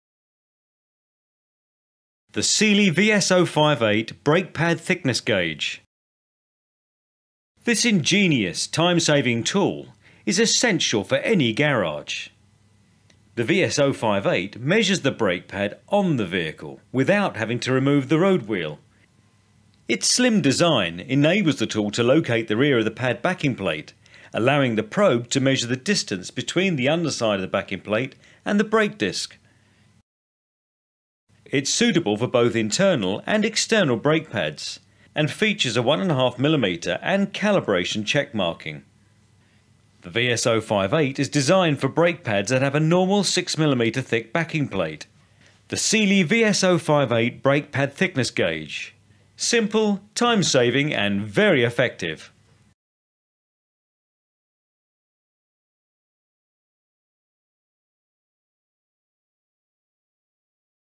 VS058 Sealey Brake Pad Thickness Gauge
Sealey Brake Pad Thickness Gauge

A brilliant and clever tool for saving time in your garage or fast fit centre!
This unique tool measures the brake pad lining on the vehicle without actually having to remove the wheel.
Its slim design allows the tool to locate the rear of the brake pad backing plate, enabling the probe to measure the distance between the underside of the backing plate and the brake disc.
Suitable for both internal and external brake pads and features a 1.5mm and calibration check marking.
Range: 1.5-13mm.
Designed for brake pads that have a normal 6mm thick backing plate (Class IV, V).